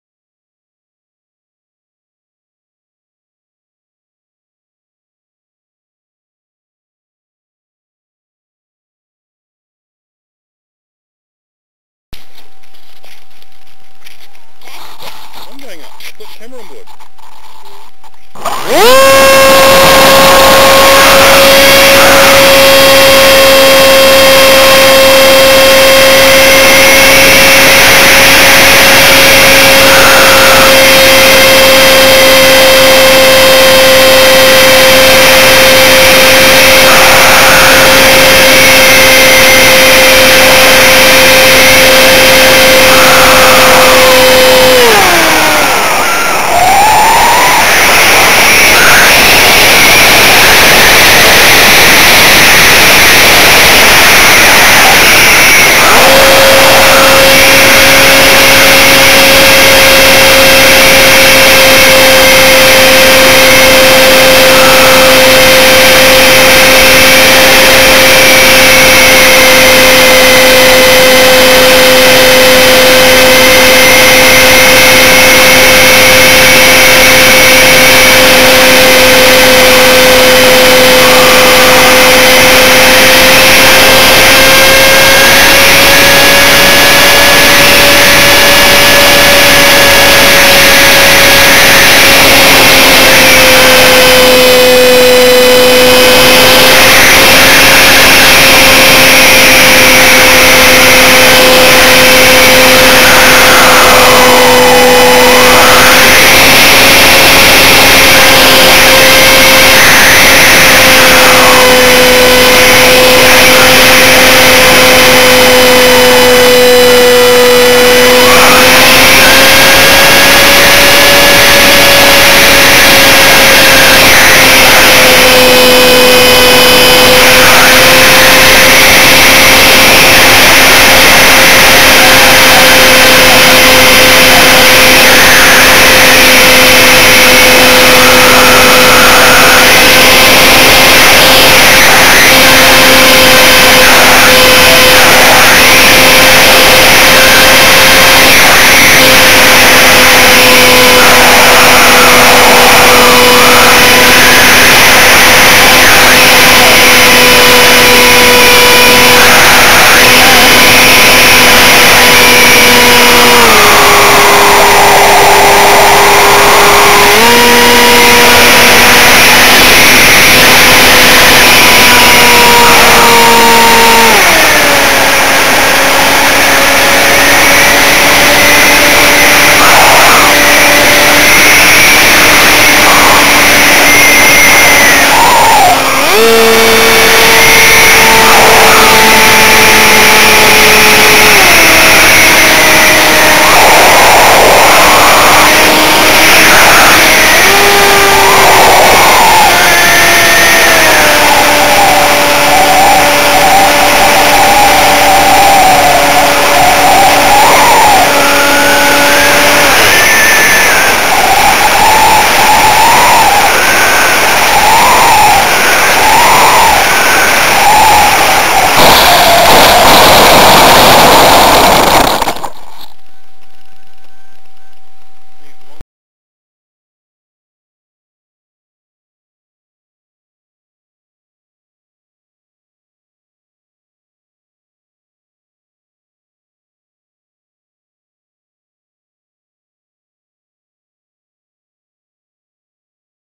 Electrafun with FCO2 mounted on nose
This vid was just a run for the cam but the son was flying as well with a slow plane and as the EF is nose heavy due to the cam its hard to chase a slow plane. did 2 high alt dives and a almost perfect landing..